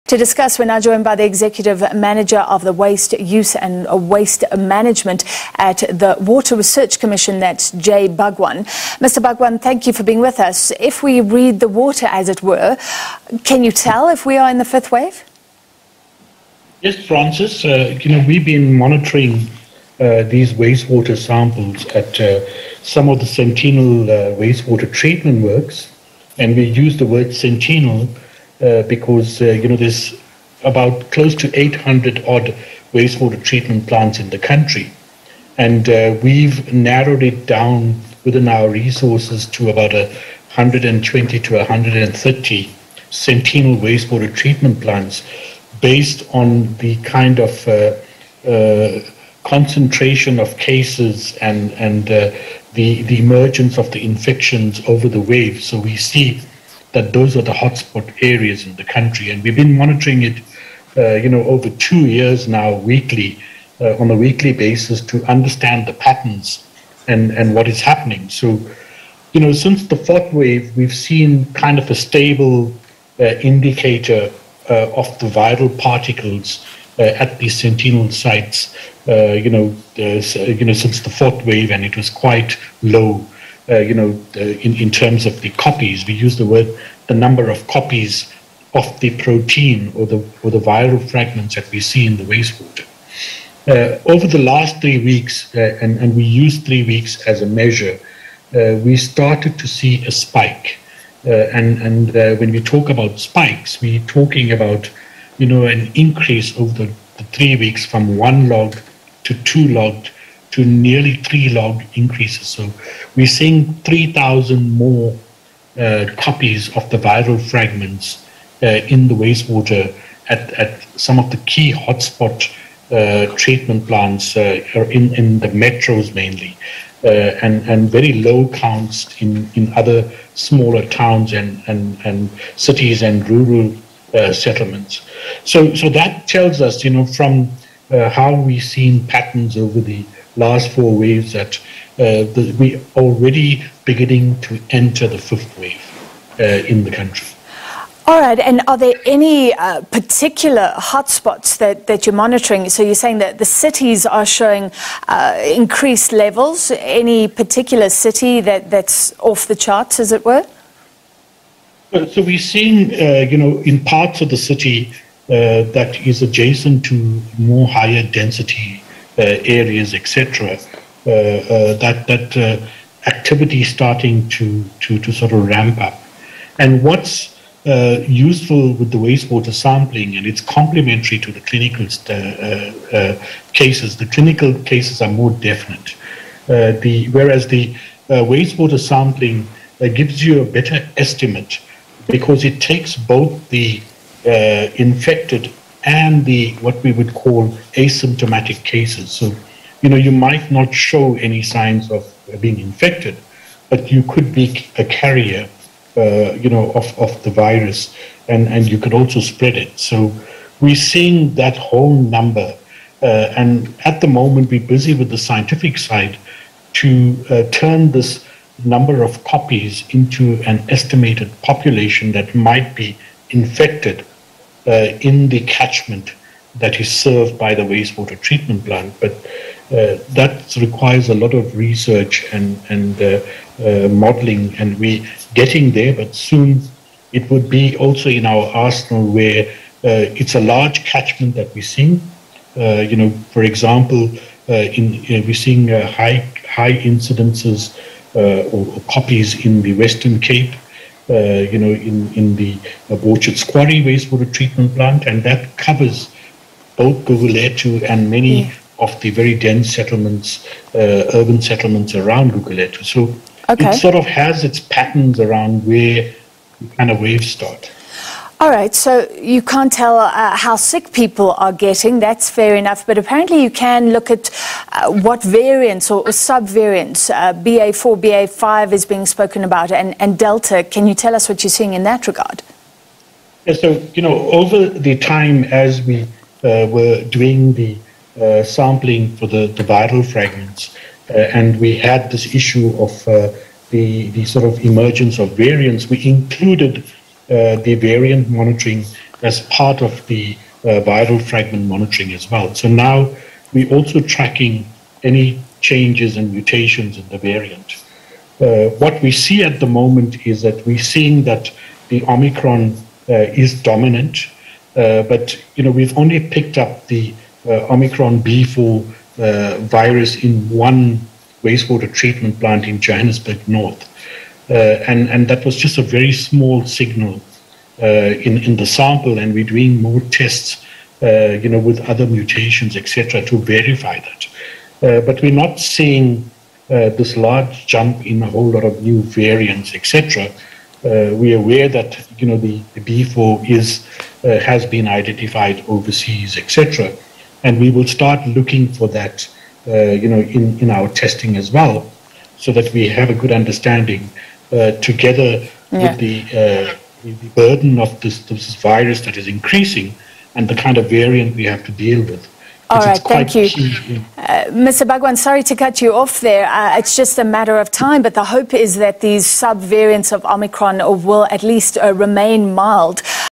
Water samples from some provinces show high levels of Corona Virus
COVID-19 infections in South Africa have jumped up dramatically since Easter.
From below 100 per day before the long weekend, South Africa reported more than 5000 infections yesterday. And at least one expert - Ridhwaan Suliman from the CSIR - believes that we have now entered the 5th wave and cases will definitely continue to rise. Well waste-water samples in various provinces are also showing signs of higher levels of COVID-19.
Wastewater is being screened for viruses to help inform South Africa's disease control efforts.

To discuss this, we're joined by the Executive Manager of the Water Use and Waste Management at the Water Research Commission.Jay Bhagwan